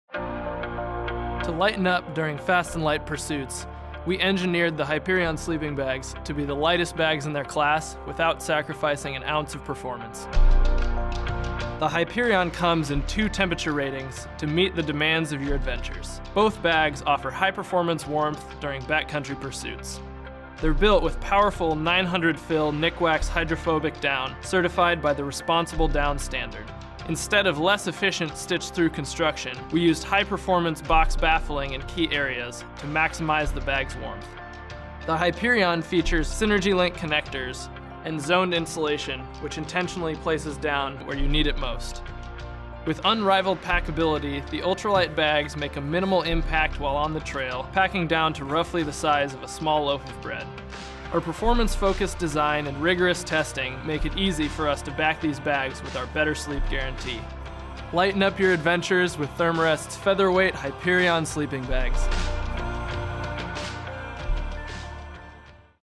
Therm-a-Rest Hyperion™ Sleeping Bag
Providing high-performance warmth at a minimal weight, the Hyperion sleeping bags are some of the lightest sleeping bags in their class without sacrificing an ounce of performance.

The Hyperion 20F/-6C and Hyperion 32F/0C combine unrivaled packability and featherweight construction with high-quality materials and essential warmth. Using 900-fill Nikwax Hydrophobic Down and box baffled construction, these quilts are backed with our Better Sleep Guarantee.